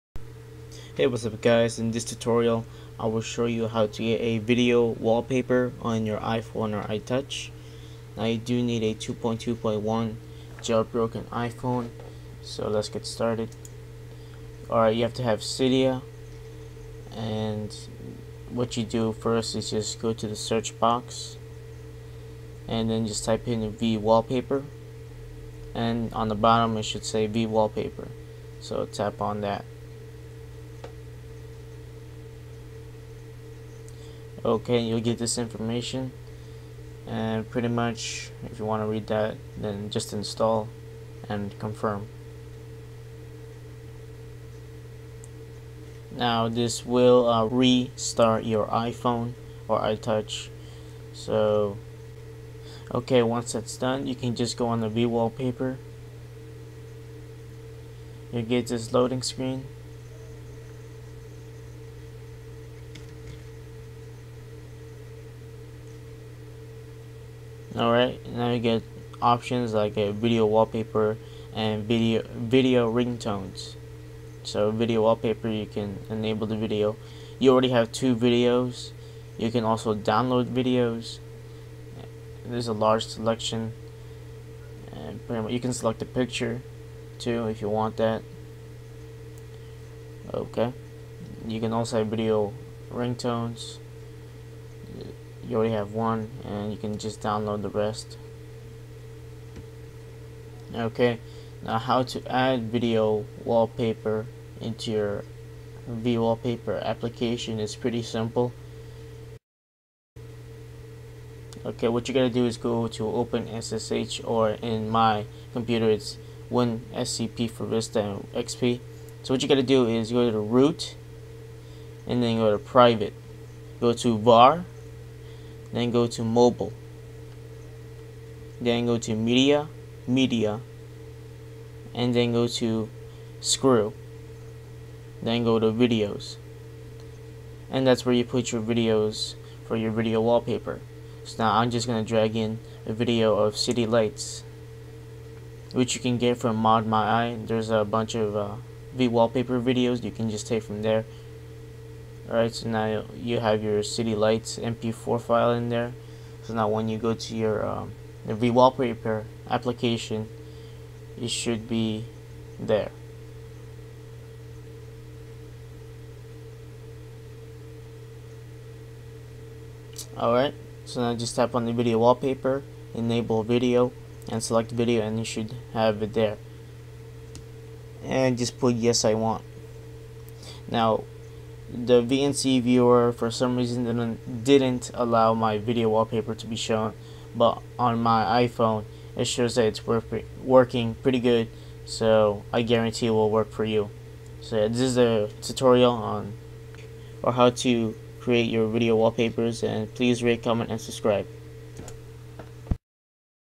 How to get a Video Wallpaper as your Background on your iPhone and iTouch
In this tutorial i will show you have to use vWallpaper. vWallpaper is an App from Cydia which lets you use videos as your wallpaper for your iPhone and iTouch